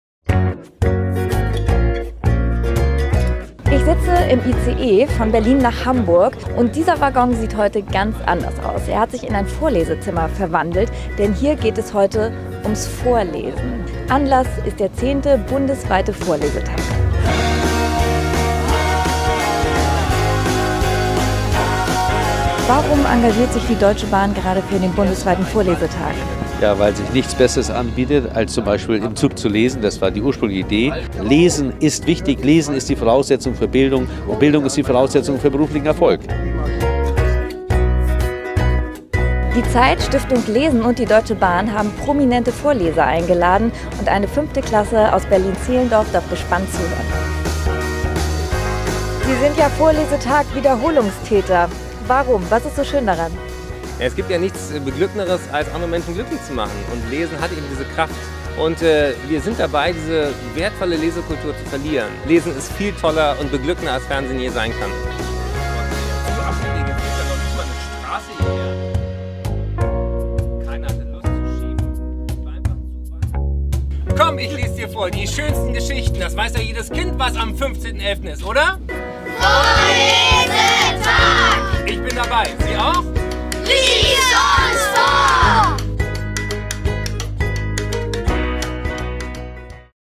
Auftakt zum 10. Bundesweiten Vorlesetag im ICE
Auf der Fahrt eines ICE von Berlin nach Hamburg konnten sich Grundschüler über eine besondere Aktion freuen: Als offizieller Auftakt zum 10. Bundesweiten Vorlesetag wurde ihnen aus dem neuen Buch „Komm, ich lese dir vor!" der Stiftung Lesen, der Wochenzeitung DIE ZEIT und der Deutschen Bahn vorgelesen.

Mit auf der Lesereise waren der Vorstandsvorsitzende der Deutschen Bahn, Dr. Rüdiger Grube, der Geschäftsführer der ZEIT, Dr. Rainer Esser, der Hauptgeschäftsführer der Stiftung Lesen, Dr. Jörg F. Maas sowie die Schauspielerin Alexandra Kamp und der Autor, Moderator, Kabarettist und Arzt Dr. Eckart von Hirschhausen.

Das Vorlesebuch „Komm, ich lese dir vor!" kann bestellt werden. Der Verkaufserlös des Buches kommt der Stiftung Lesen zugute.